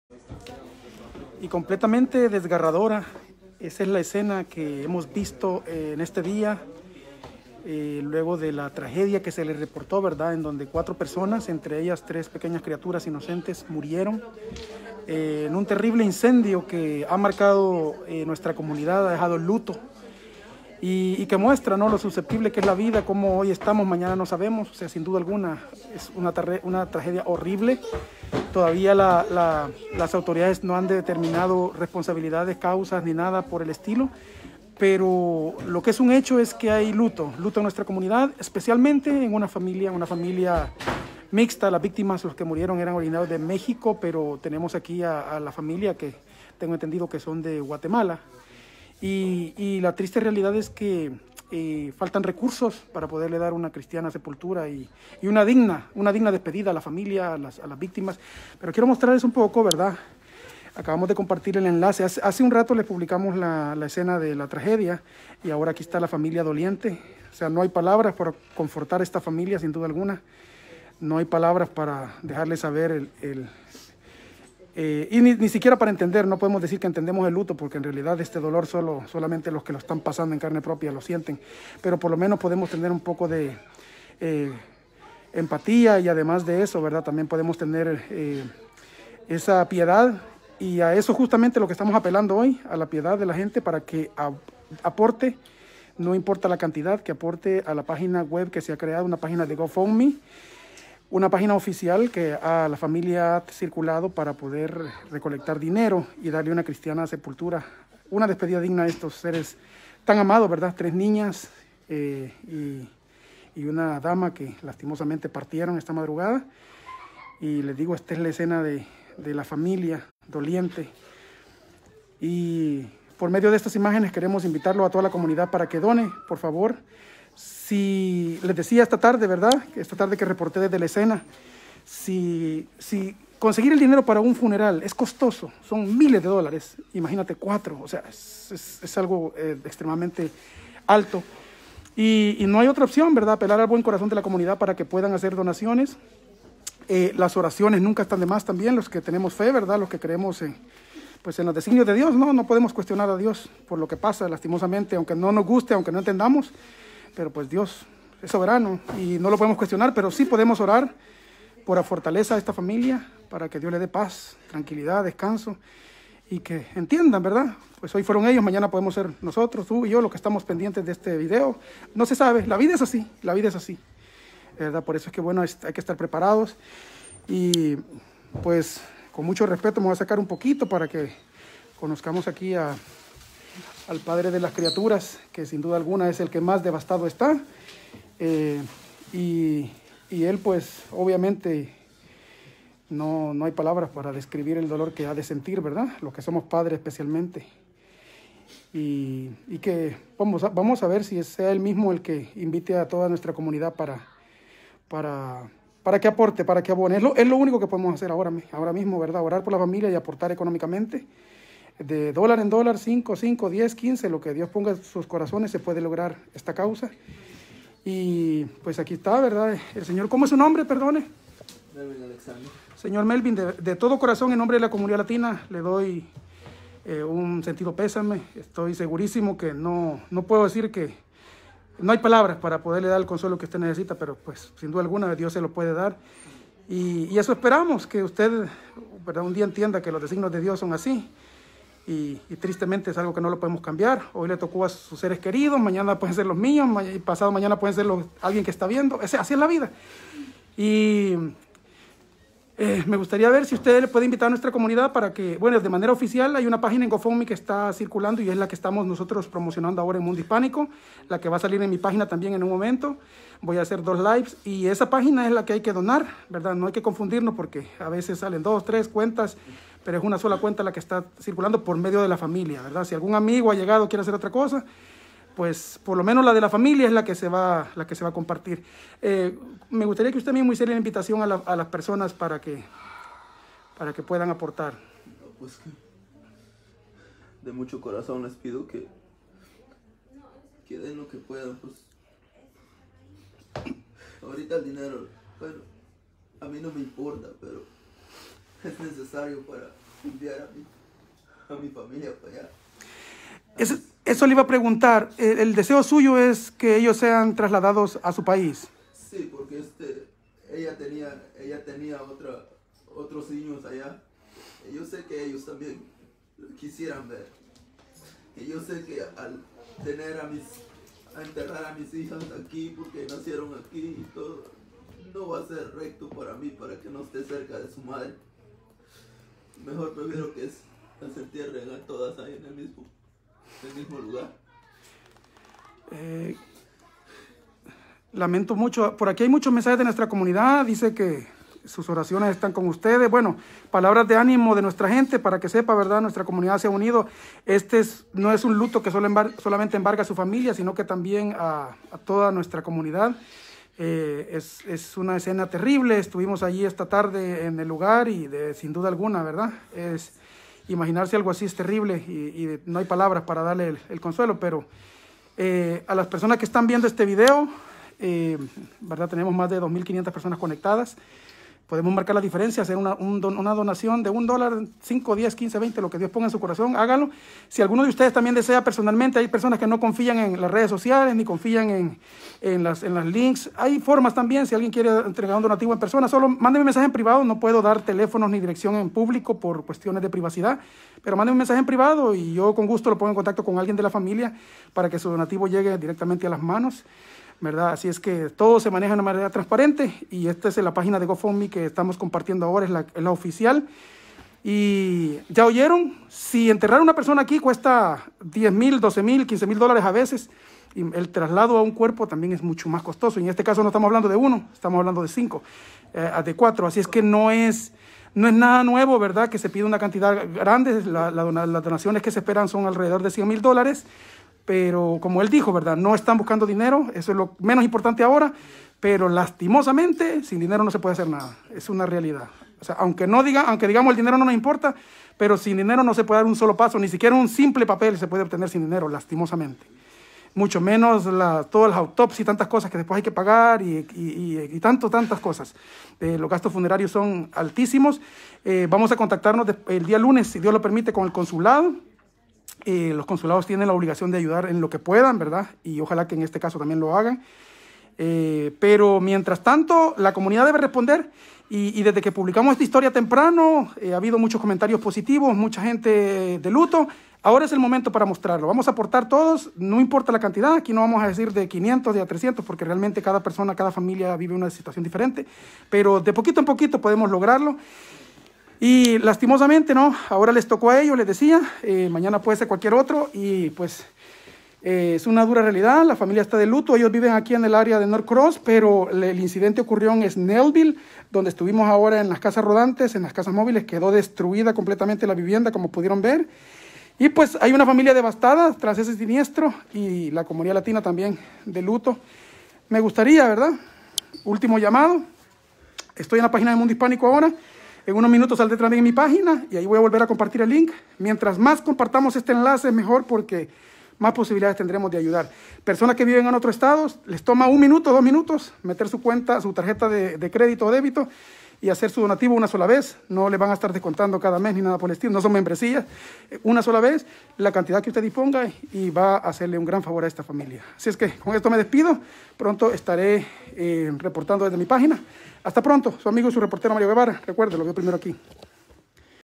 Hispano pierde a su familia en un incendio y pide ayuda para su funeral | Noticias
Las autoridades no han podido revelar la identidad de las víctimas, pero medios locales informaron que el padre de las menores fue el que las identificó: La madre era María Isabel Martínez, y las pequeñas Alexa, de seis años; Ashley, de cuatro, y Daniela de apenas un año de edad.

De acuerdo con testigos, la madre era originaria de México y tenía tres años de estar viviendo en la casa, que estaba ubicada en un barrio de casas móviles donde la mayoría de los residentes son hispanos.

El padre de las pequeñas, que se identificó como Alexander Díaz, pidió ayuda a la comunidad para poder hacer frente a los gastos del funeral de las cuatro, por lo que abrió una cuenta de Gofundme.